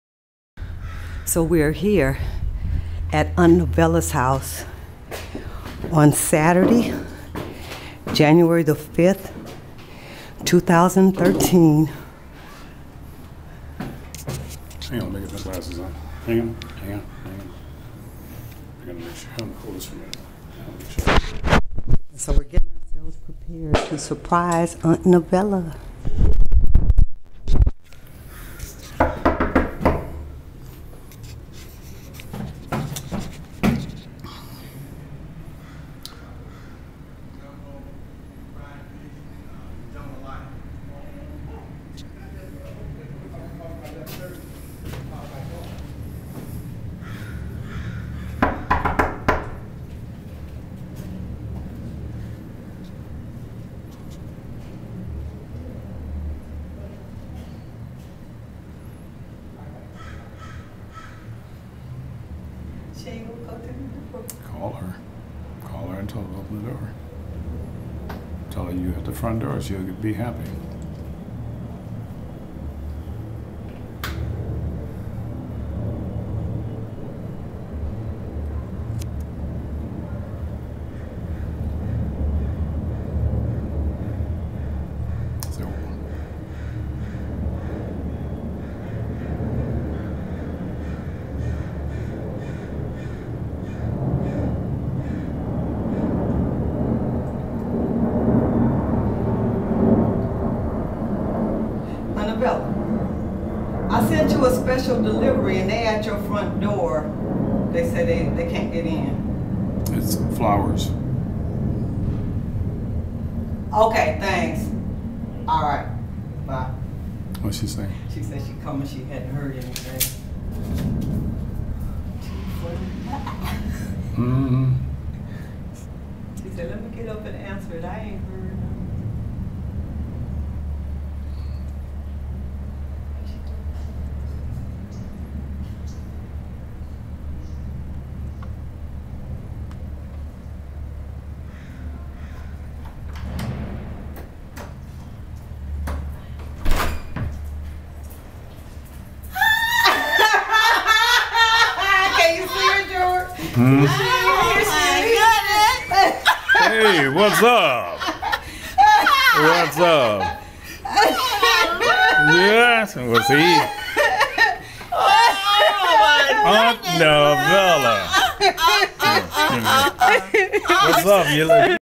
Making Memories 2013
Aunt Novella haven't seen in 18 years.. SURPRISE VISIT..